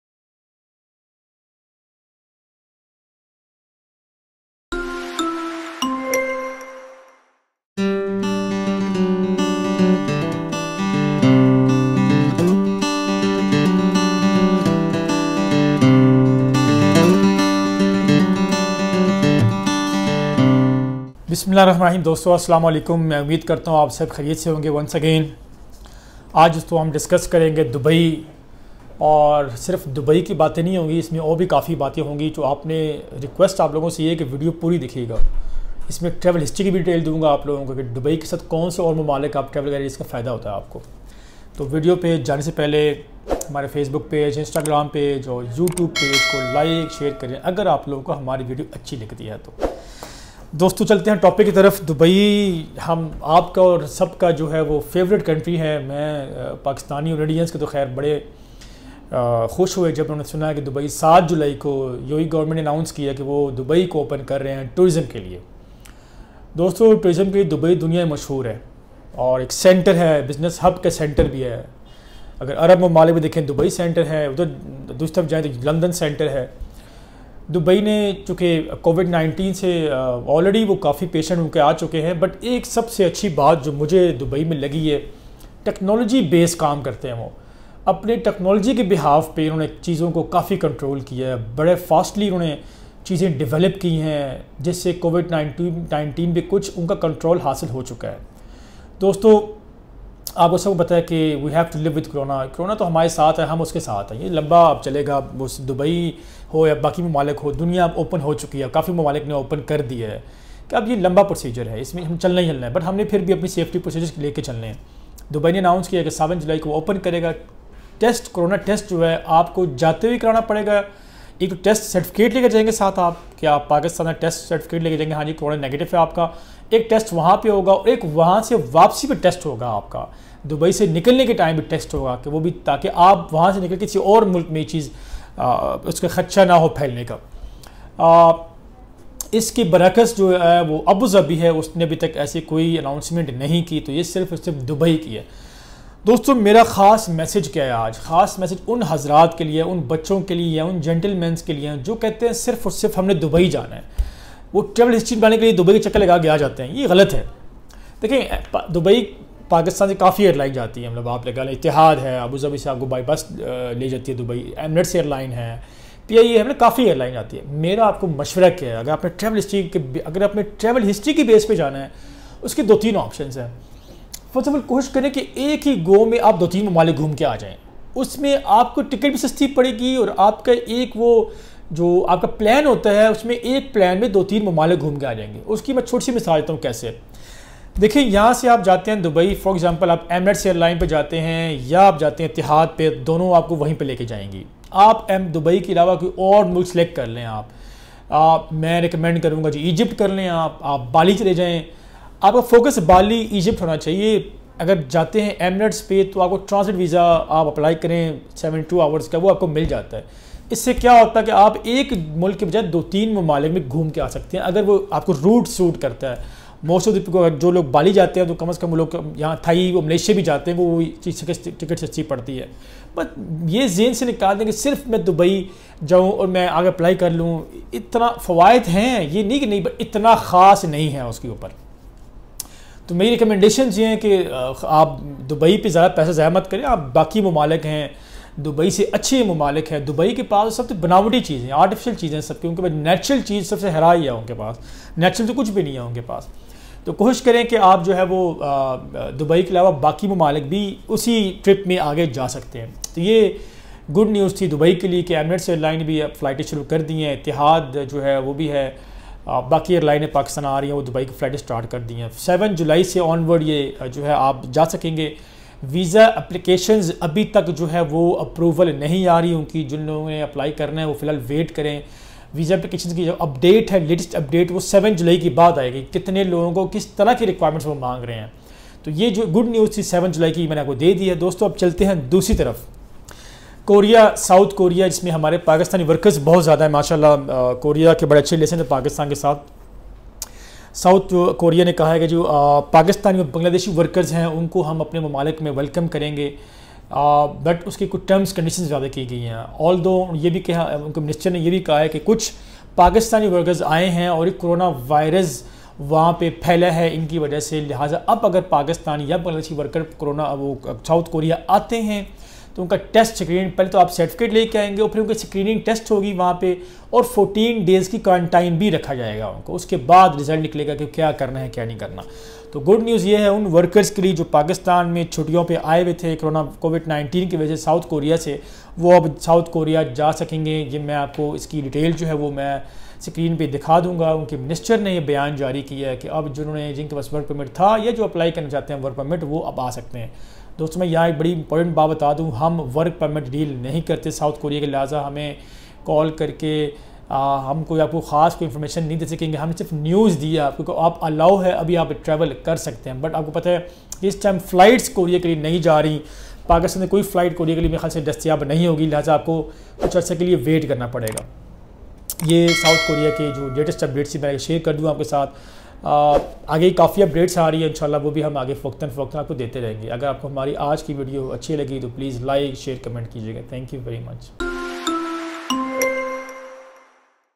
Dubai Tourism & South Korea Visa details|دبئی اور ساؤتھ کوریا سے ویزا کی تازہ ترین معلومات
I will explain some details regarding opening of tourism in Dubai, and whats the best time to travel to Dubai and i suggest to add some more countries while you visit Dubai, it will increase your travel history.
i will also add some good news from south Korea work permit, for Pakistanis and Bangladesh workers.